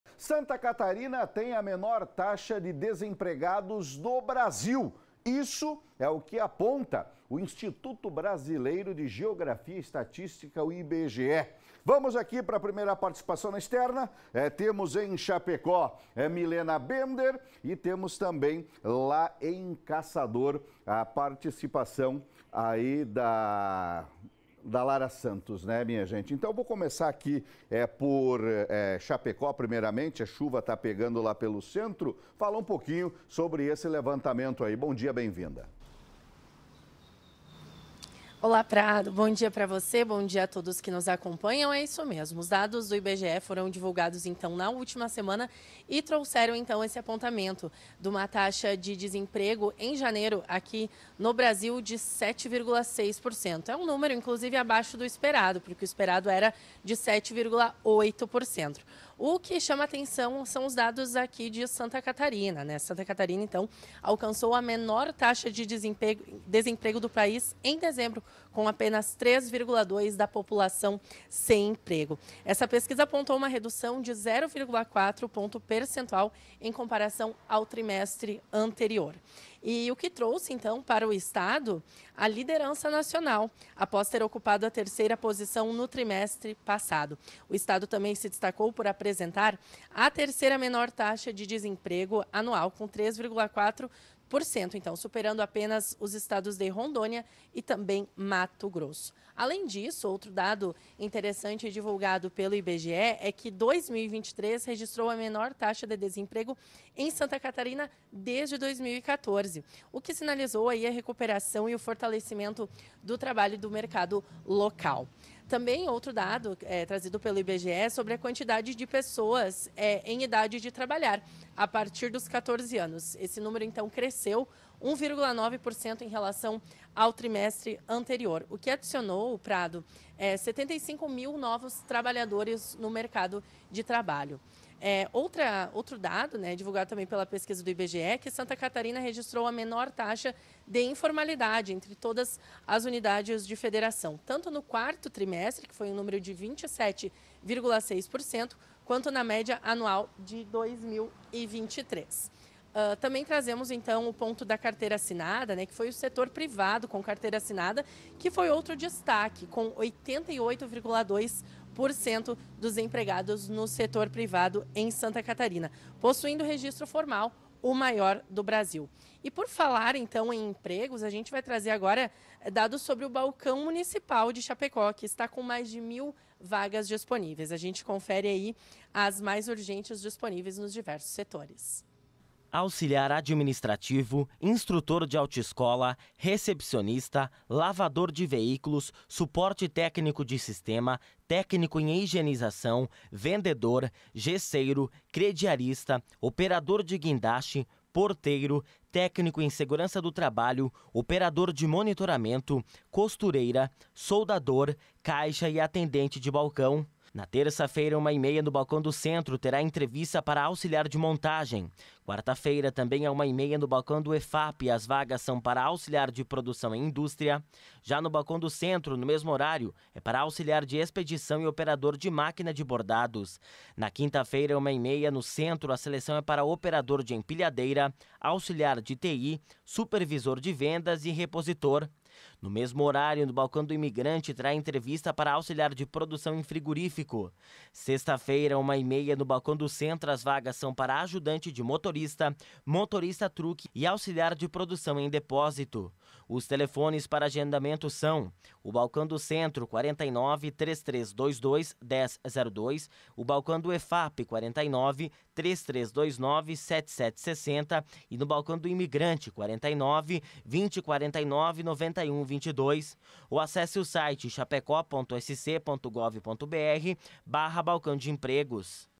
Santa Catarina: Liderando o Mercado de Trabalho Brasileiro
Com base nos dados recentes do Instituto Brasileiro de Geografia e Estatística (IBGE), Santa Catarina emerge como líder nacional na criação de empregos, apresentando a menor taxa de desemprego em todo o país. Esses números impressionantes refletem não apenas a resiliência econômica do estado, mas também suas políticas públicas eficazes e seu ambiente favorável aos negócios.